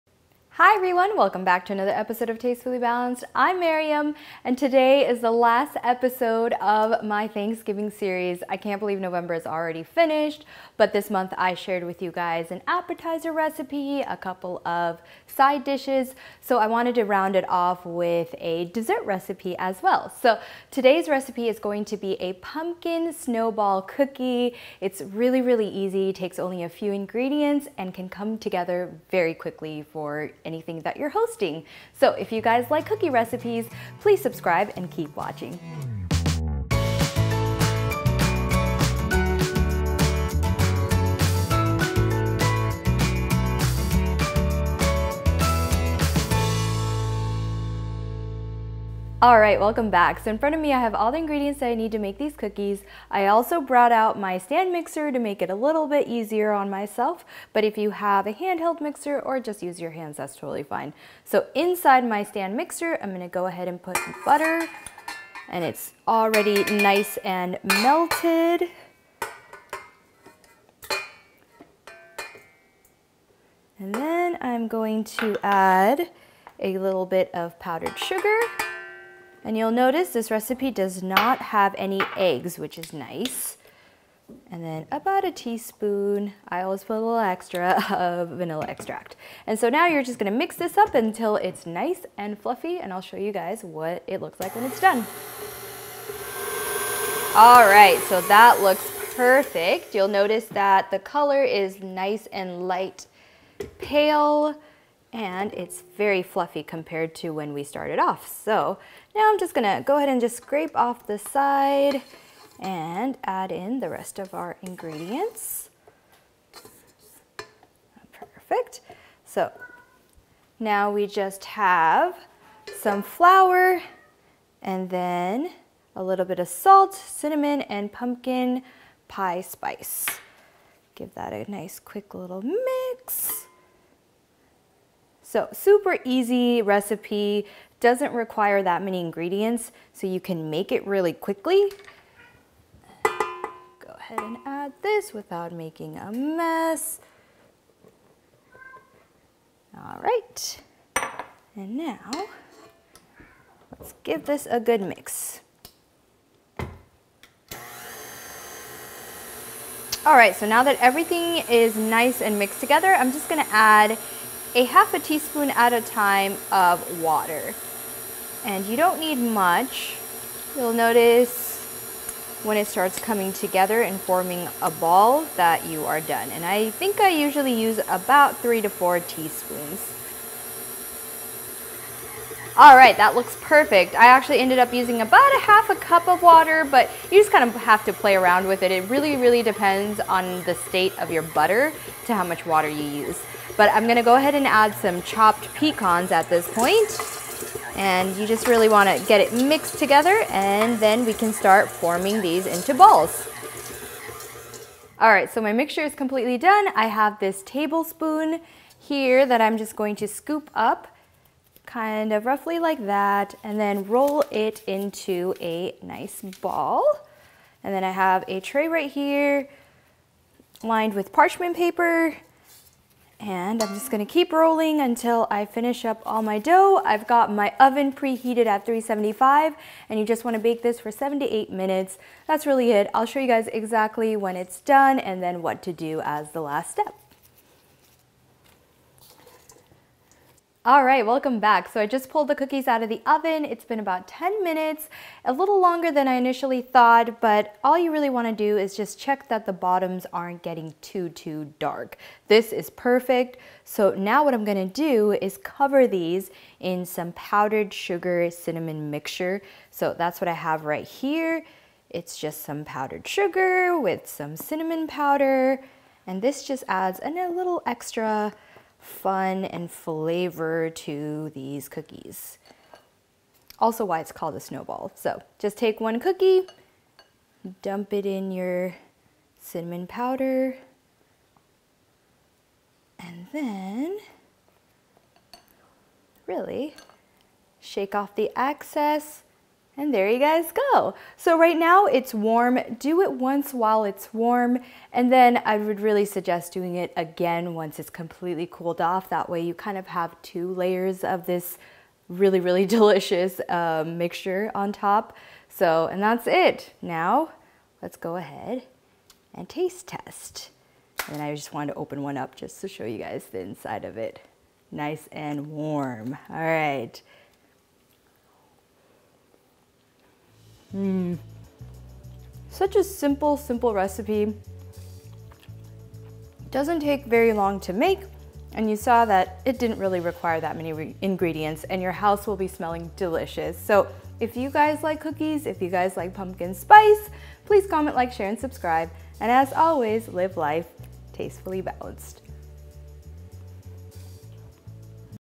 Pumpkin Snowball Cookies | Thanksgiving Series!
Need a recipe for a quick dessert at your next fall gathering? Have a cookie exchange? These Pumpkin Snowball Cookies are so easy and fun to make! Perfect for kids as there are no eggs involved. I love making these to give away at my parties!